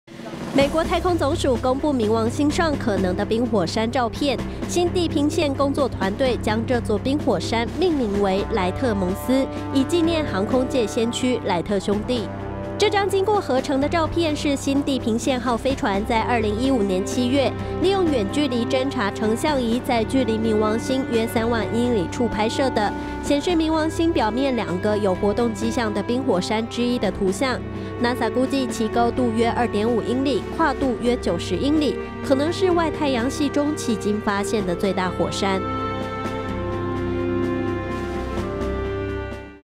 外太陽系最大火山 NASA發冥王星照片【大千世界】冰火山｜美國太空總署｜新地平線｜萊特兄弟｜Wright Mons
【新唐人訊】美國太空總署公佈冥王星上可能的冰火山照片。新地平線工作團隊將這座冰火山命名為「萊特·蒙斯」(Wright Mons)，以紀念航空界先驅萊特兄弟。